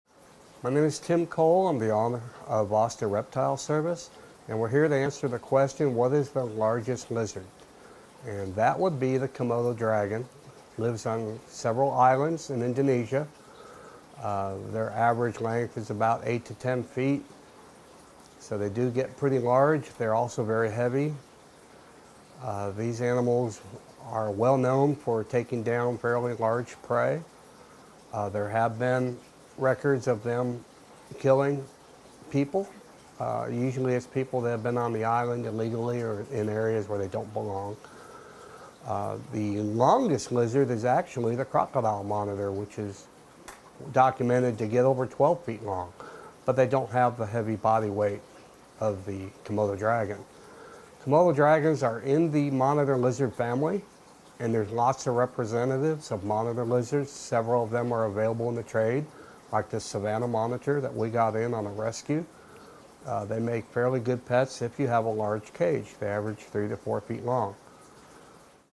Reptiles & Snake Identification : What's the Largest Lizard?
The largest lizard in the world is the Komodo dragon, which lives on several islands in Indonesia and averages 8 to 10 feet in length. Learn about the heavy body weight of Komodo dragons, which allow them to take down large prey, with help from an advanced certified animal control officer in this free video on lizards and reptiles.

Expert: Tim Cole
Bio: Tim Cole is the owner of Austin Reptile Service, and has been keeping reptiles for over four decades.